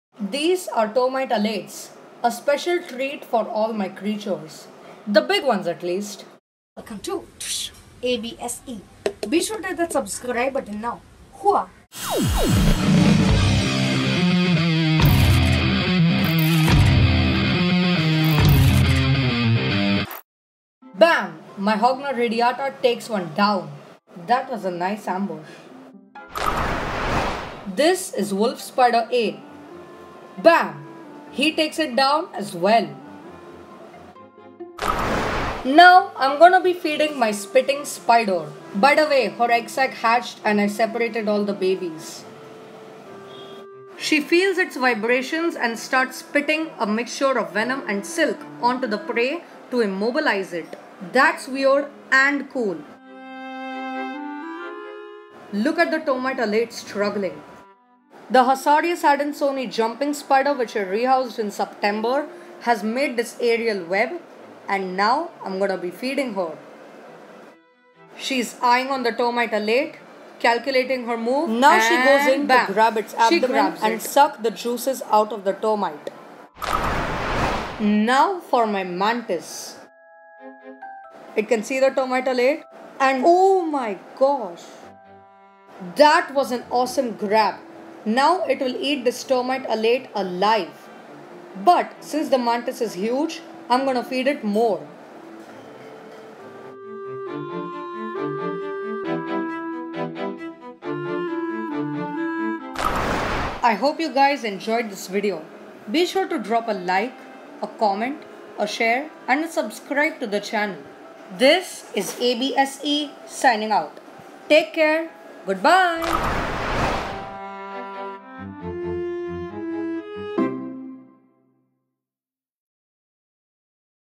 TREATS for my ANIMALS! Feedvid (A.B.S.E.)
Feedvid Creatureverse A.B.S.E.

Today on A.B.S.E., inside the Creatureverse, I shall feed some very cool animals of mine with protein-packed termite alates (a treat for them). This video features my Spitting Spider, mantis, wolf spiders and more!!! Watch this feeding video till the end to find out more...

Season 2